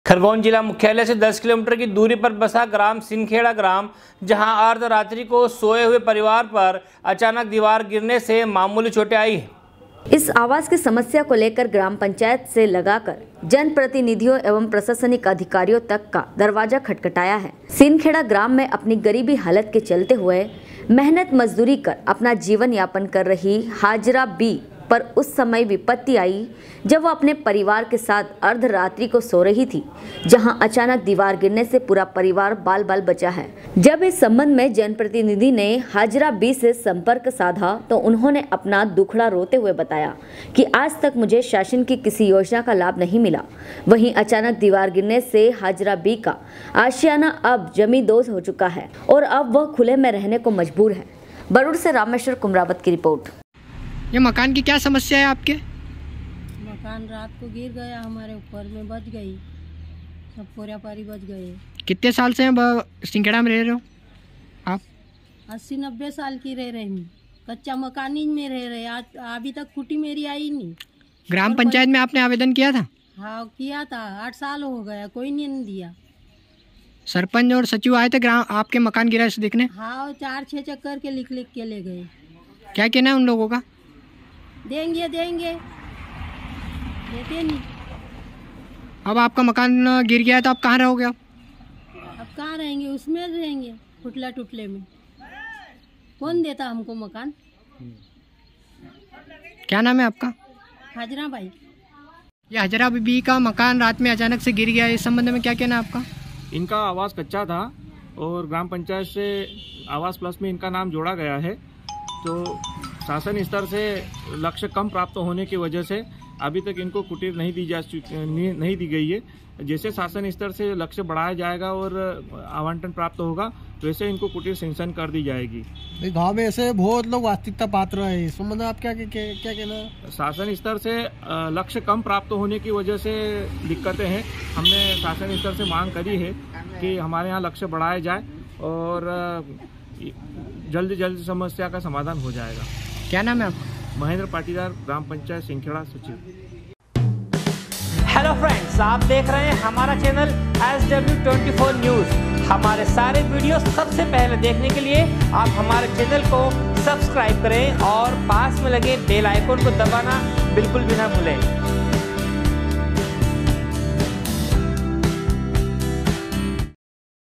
सिंनखेड़ा ग्राम जहां अर्ध रात्रि को सोए हुए परिवार पर अचानक दीवार गिरने से मामूली चोटें आई हे,
खरगोन जिला मुख्यालय से 10 किलोमीटर की दूरी पर बसा ग्राम सिंनखेड़ा ग्राम जहां अर्ध रात्रि को सोए हुए परिवार पर अचानक दीवार गिरने से मामूली चोटें आई हे,